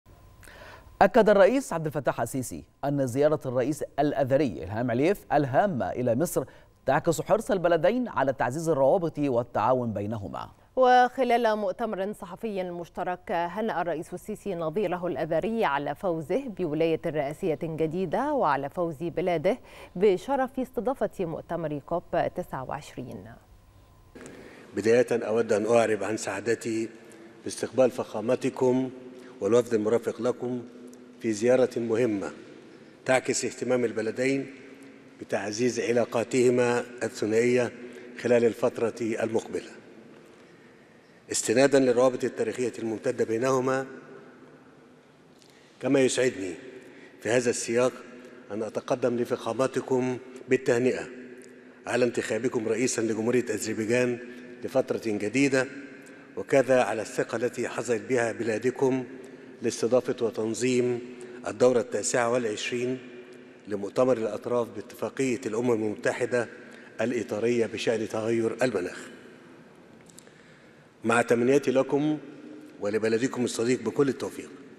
الرئيس السيسي: زيارة الرئيس الأذري العامة لمصر تعكس حرص البلدين على تعزيز الروابط والتعاون بينهما
نشرة السادسة | الرئيس السيسي: زيارة الرئيس الأذري العامة لمصر تعكس حرص البلدين على تعزيز الروابط والتعاون بينهما